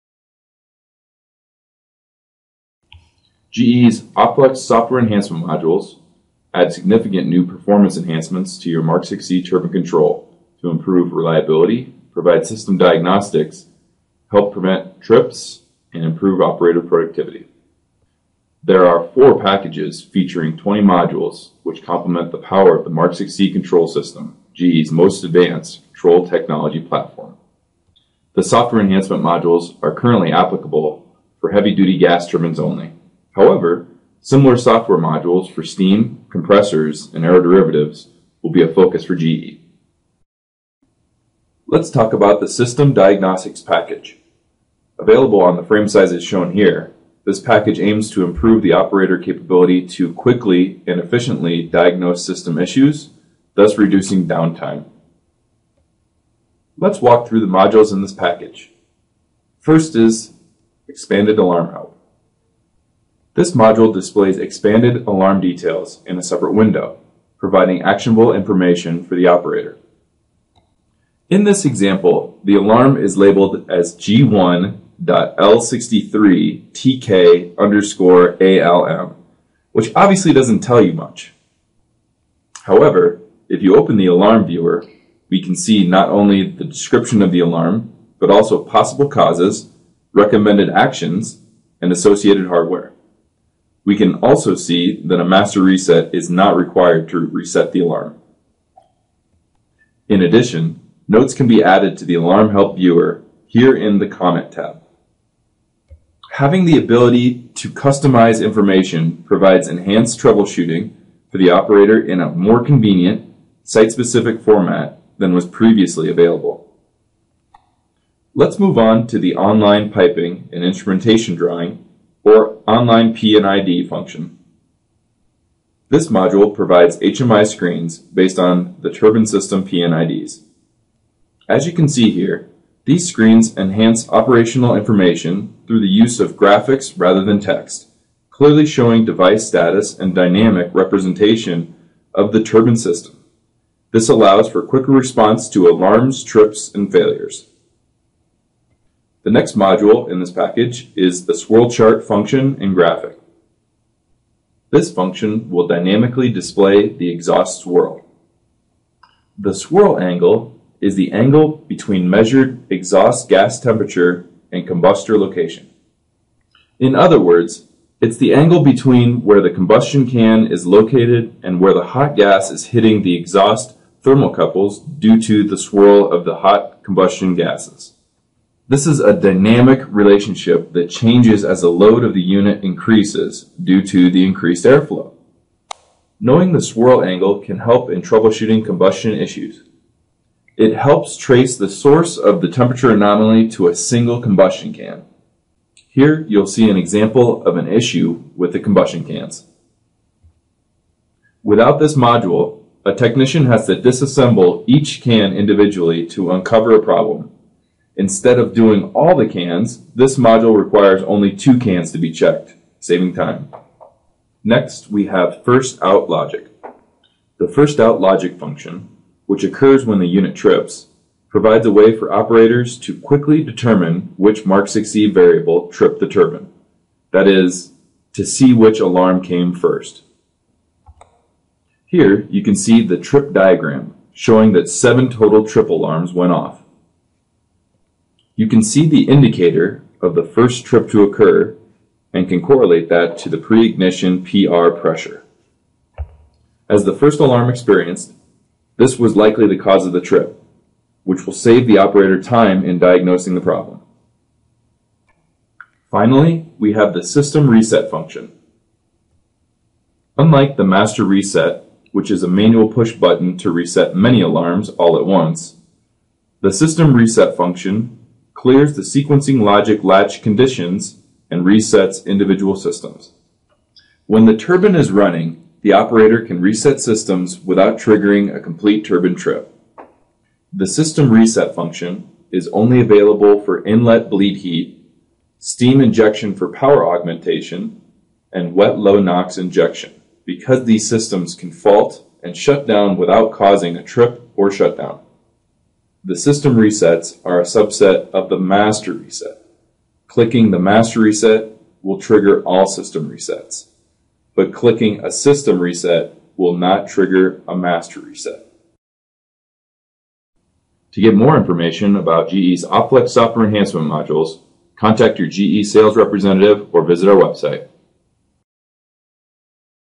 OpFlex SEM Demo: System Diagnostics Package
Learn more about GE’s four OpFlex packages, featuring 20 modules that complement the power of the Mark Vle control system.